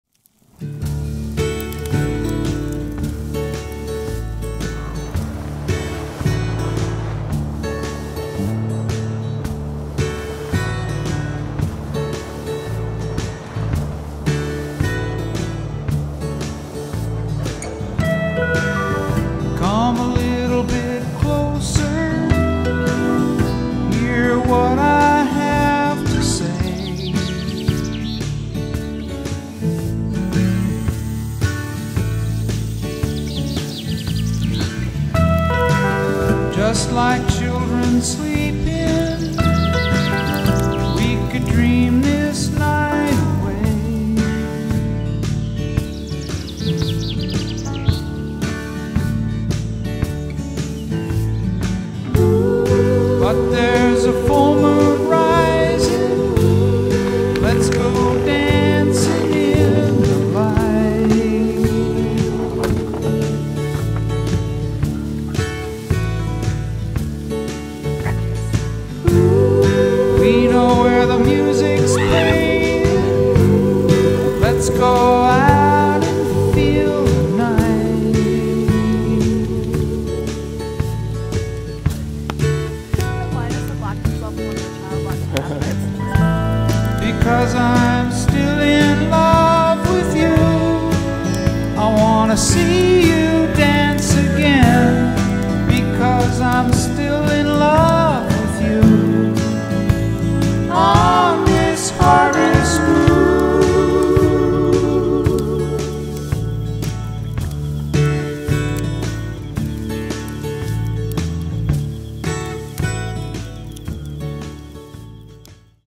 Scamping
Short camping trip to Steamboat Springs to try out a Scamp! Found a great disbursed spot and had a great day on some amazing single track at Emerald Mountain.